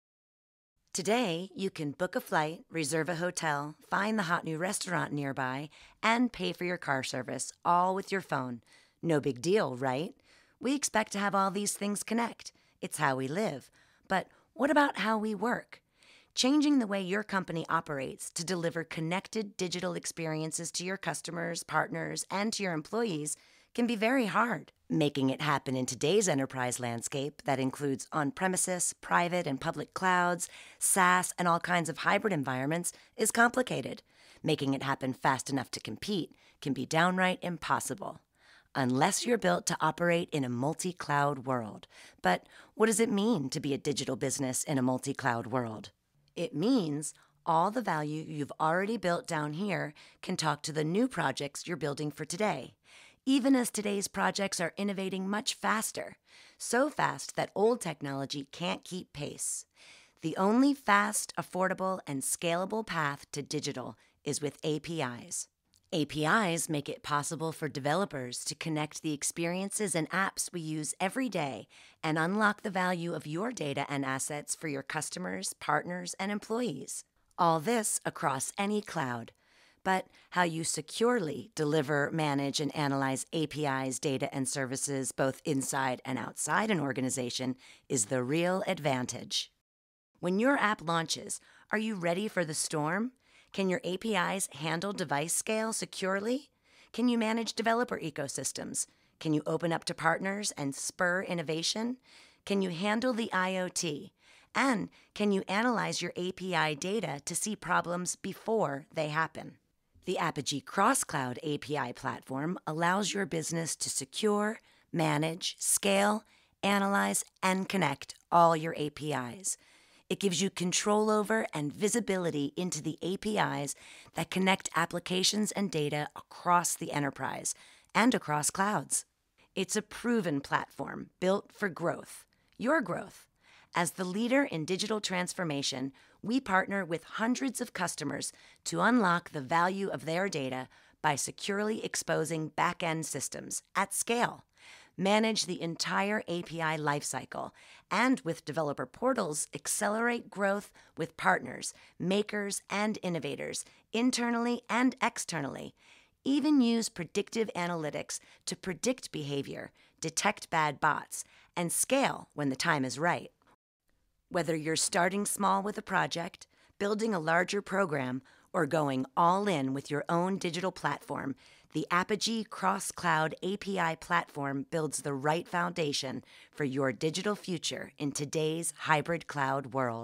The Apigee Cross-Cloud API Platform
In this quick explainer video, learn how the Apigee cross-cloud API platform helps businesses build the right foundation for the digital future in today's hybrid-cloud world.

To learn more about how the Apigee platform compares to other providers in the industry, download the 2018 Gartner Magic Quadrant for Full Lifecycle API Management. This must-read for IT professionals provides a comprehensive analysis of the API management marketplace.